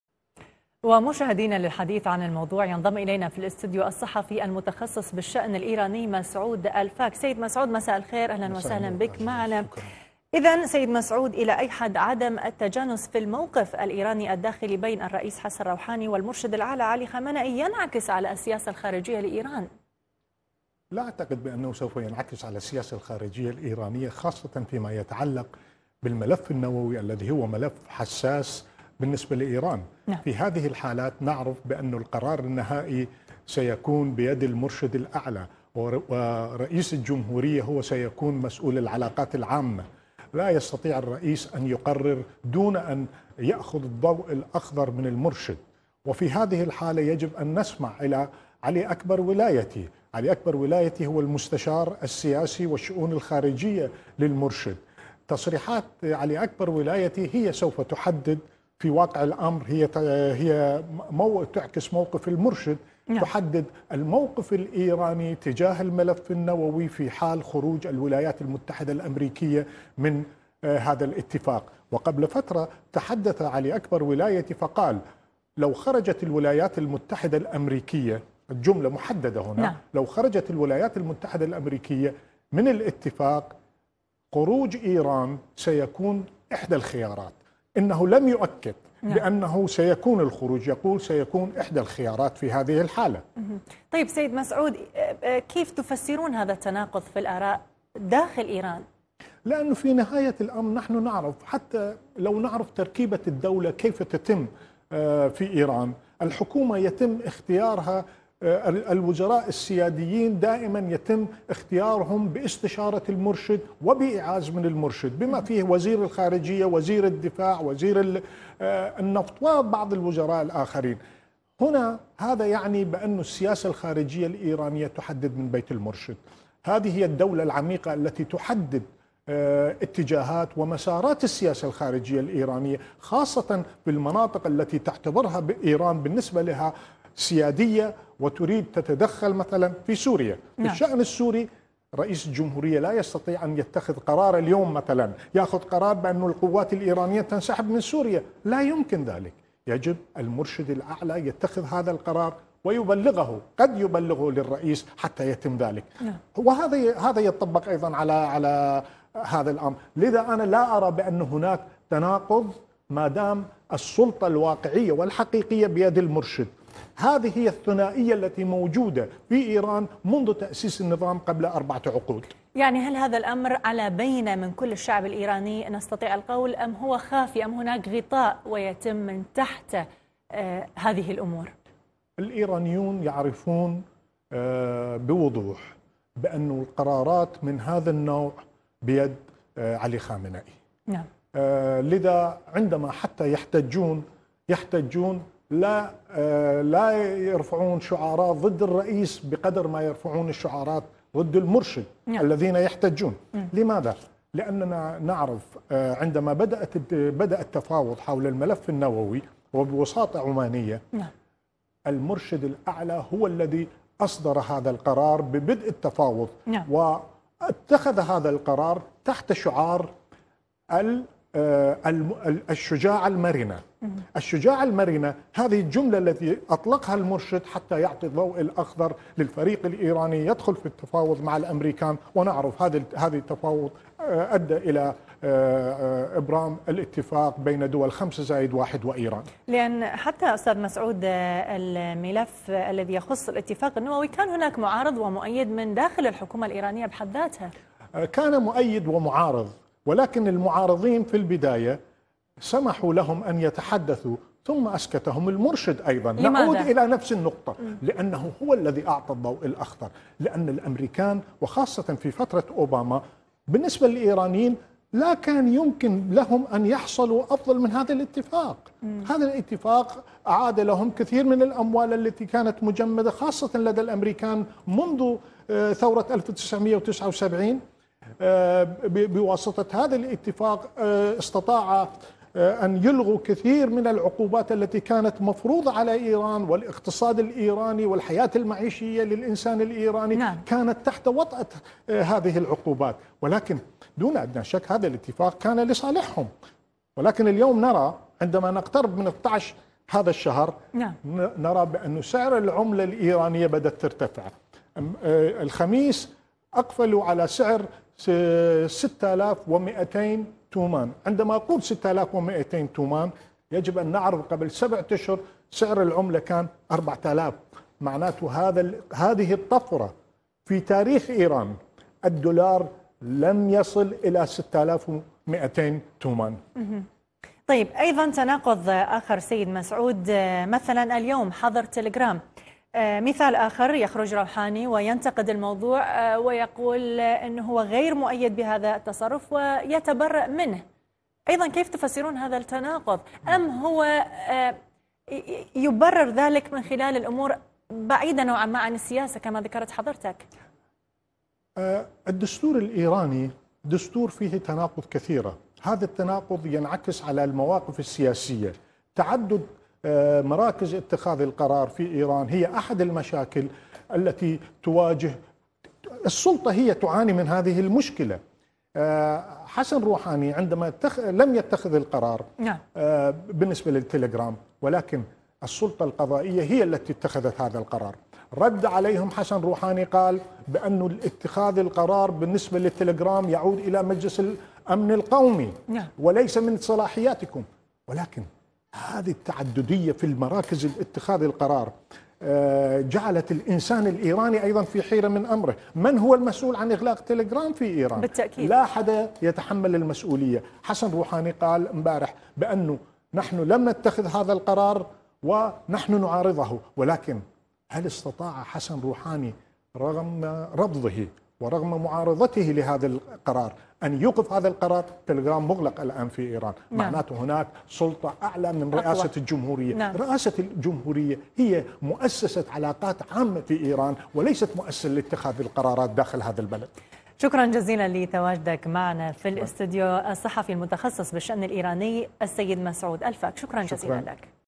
شرح مفصل: مؤشرات في إيران على عدم تجانس بين سلطات النظام
ينضم الينا في الاستديو الصحفي المتخصص بالشأن الايراني مسعود الفاك